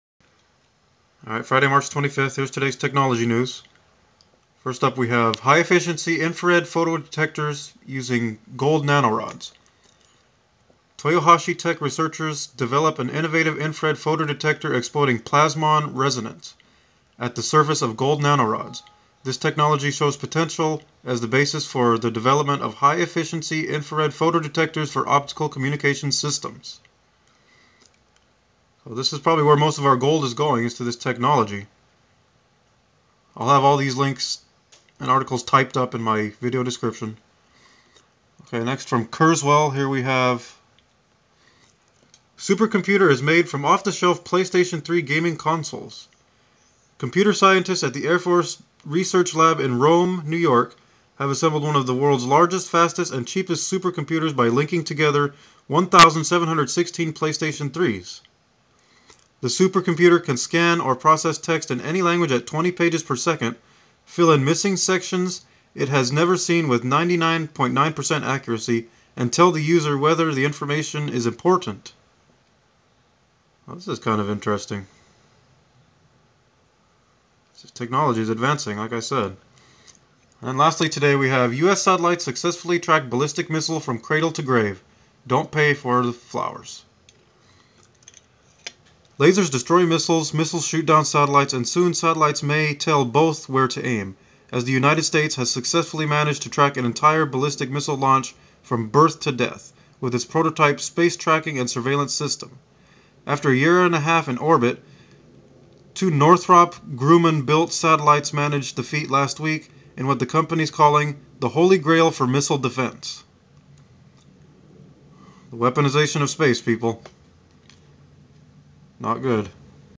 3-25-2011 Technology News: Supercomputer made from off-the-shelf PlayStation 3 gaming consoles
From Physorg, High efficiency infrared photodetectors using gold nanorods:

Toyohashi Tech researchers develop an innovative infrared photodetector exploiting 'plasmon resonance' at the surface of gold nanorods, which enhances the density of photoelectrons excited over the Schottky barrier. This technology shows potential as the basis for the development of high efficiency infra-red photodetectors for optical communications systems.

Devices used for the detection of light and other forms of electromagnetic energy include calorimeters, superconducting devices, and photodiodes used in optical communications systems.

Now, typical semiconductor devices include Schottky barrier photodetectors—where a PN junction is not necessary. However, for optical communications systems applications, it is necessary to improve the photo detection efficiency in the 1.3~1.5 micrometer range of wavelengths.

Here, Mitsuo Fukuda and colleagues used the localized surface plasmon (LSP) effects exhibited by gold nano-rods to improve the optical response of Schottky photodiodes. Notably, the desired resonance wavelength can be obtained by appropriate choice of the dimensions of gold nanorods. Thus combining Schottky barriers with gold nanorods holds promise as a means of producing high efficiency photodiodes.

From Kurzweil, Supercomputer is made from off-the-shelf PlayStation 3 gaming consoles:

Computer scientists at the Air Force Research Lab in Rome, NY, have assembled one of the world's largest, fastest, and cheapest supercomputers by linking together 1,716 PlayStation 3s, says Mark Barnell, director of high-performance computing at the Air Force Research Lab.

The supercomputer can scan or process text in any language at 20 pages per second, fill in missing sections it has never seen with 99.9 percent accuracy, and tell the user whether the information is important.

Video processed from radar signals, including ground-based radar images of space objects, can be viewed in real time or played back to investigate what led to an event. A viewer can change perspectives, going from air to ground to look around buildings.

The supercomputer went online late last year, and it will likely change the way the Air Force and the Air National Guard enable 24-hour, real-time surveillance over a roughly 15-mile-wide area, says Barnell.

From Engadget, US satellites successfully track ballistic missile from cradle to grave, don't pay for the flowers:

US satellites successfully track ballistic missile from cradle to grave, don't pay for the flowers

Lasers destroy missiles, missiles shoot down satellites, and soon, satellites may tell both where to aim, as the United States successfully managed to track an entire ballistic missile launch from "birth-to-death" with its prototype Space Tracking and Surveillance System. After a year and a half in orbit, two Northrop Grumman-built satellites managed the feat last week, in what the company's calling "the Holy Grail for missile defense." While we're not reading about any plans to mount any lasers on the satellite's... ahem... heads, Space News reports that the US Navy will attempt to relay the satellite tracking data to its Aegis ships with interceptor missiles on board, and hopefully obliterate incoming projectiles with the extra range and reaction time that satellite coordinates afford. The Navy has reportedly scheduled its first game of space-based Missile Command for next month.